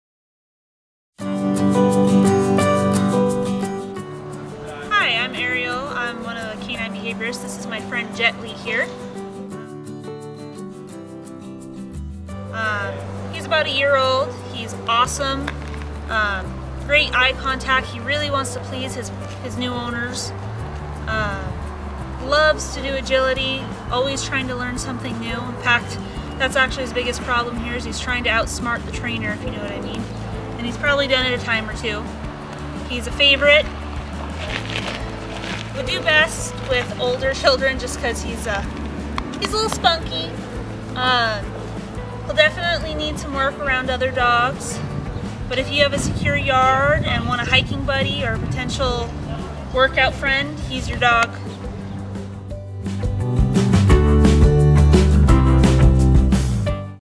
Jetly
Jetly is looking for his FUR-ever home. This spunky intelligent guy will need a fenced yard to play in and a best friend who doubles as his hiking partner. Jetly is waiting to please his new family at Yavapai Humane Society.